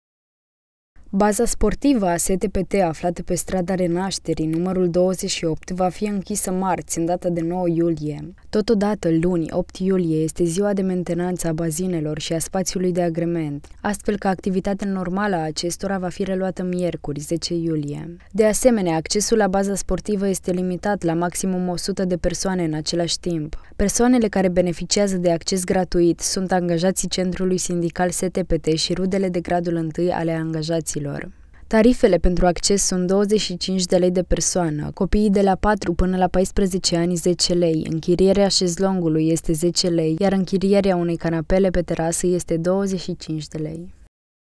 Modificări la Baza Sportivă STPT
Baza Sportivă a STPT, aflată pe strada Renașterii, numărul 28, va fi închisă marți, în data de 9 iulie. Totodată, luni, 8 iulie, este ziua de mentenanță a bazinelor și a spațiului de agrement, astfel că activitatea normală a acestora va fi reluată marți, 10 iulie. De asemenea, accesul la baza sportivă este limitat la maximum 100 de persoane în același timp.